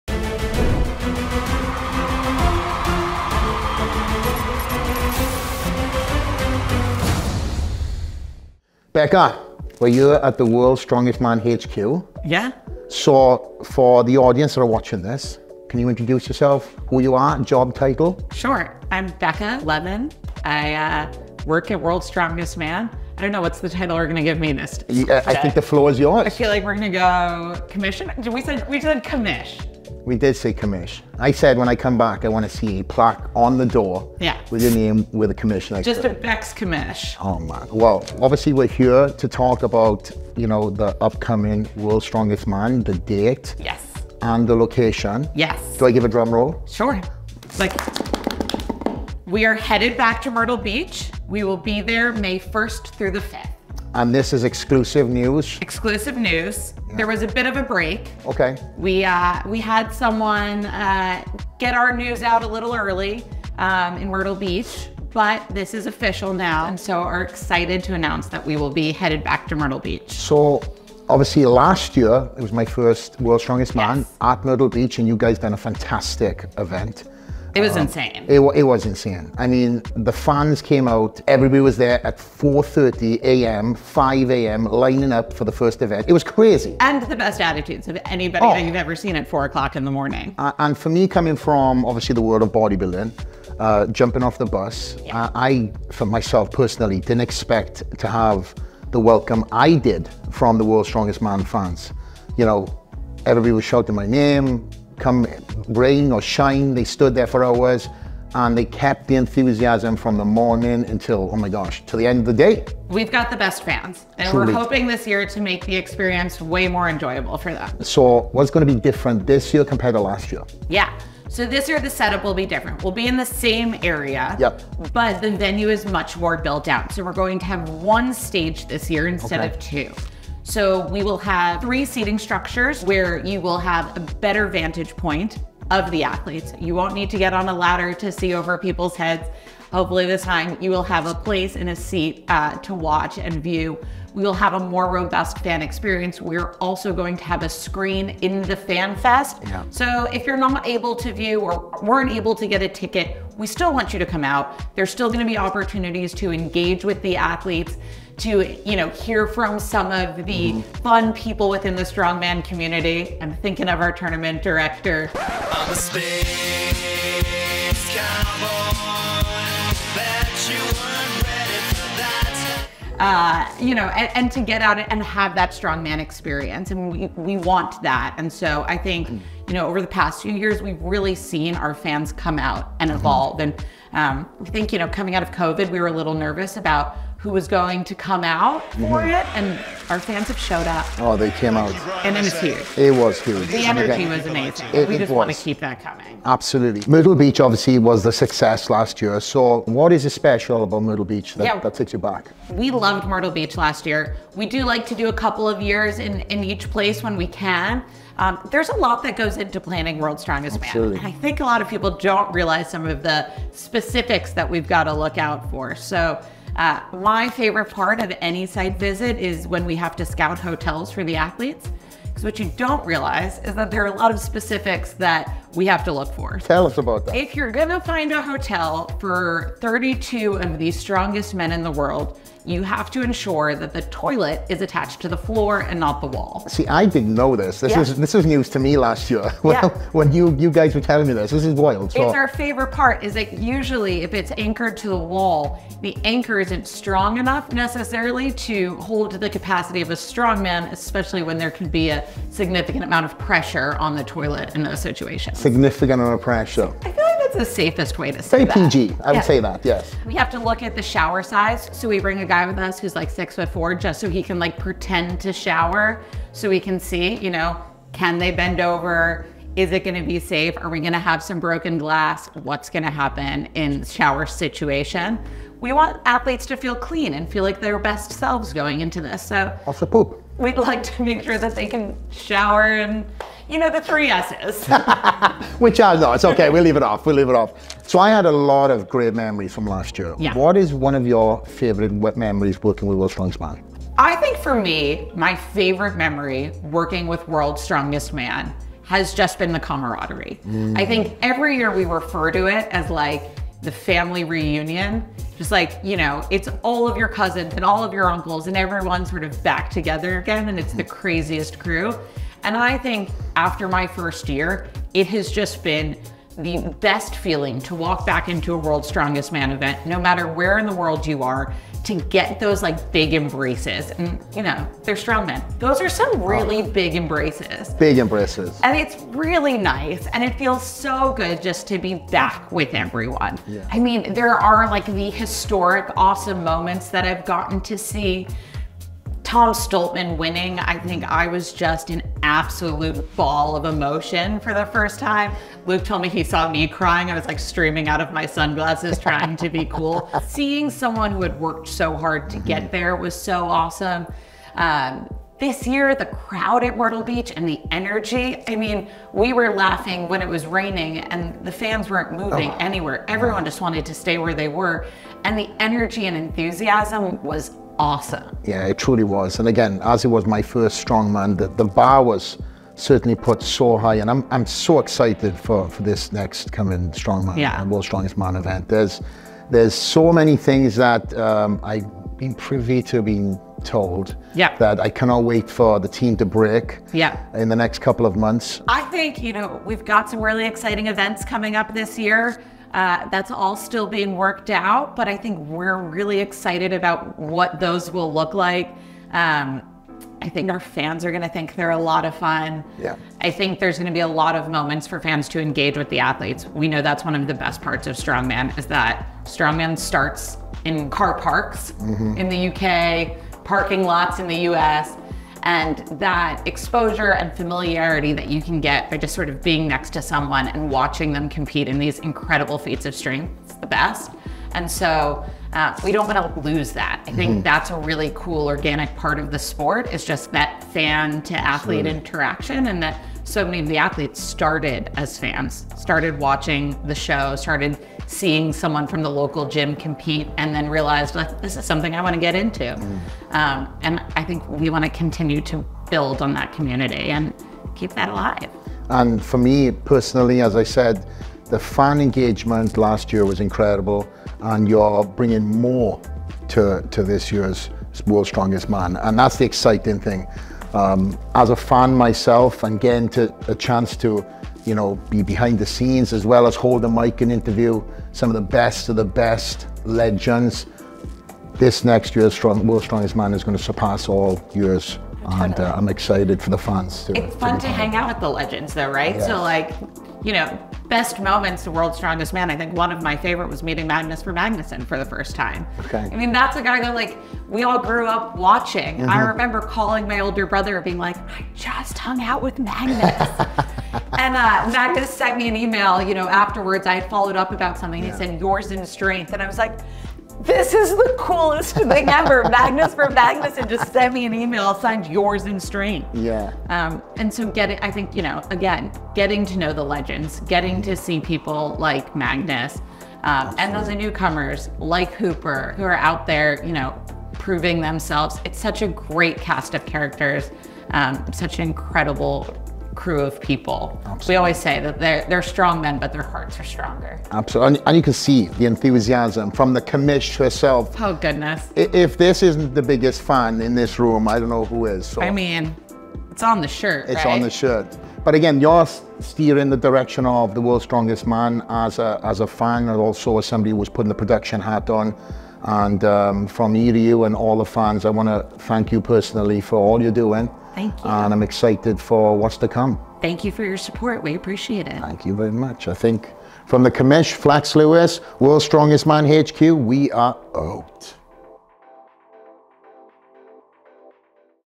2024 SBD World's Strongest Man // Date & Location Reveal
Tune in for an exclusive interview with strongman insider Flex Lewis to announce the 2024 SBD World’s Strongest Man date and location!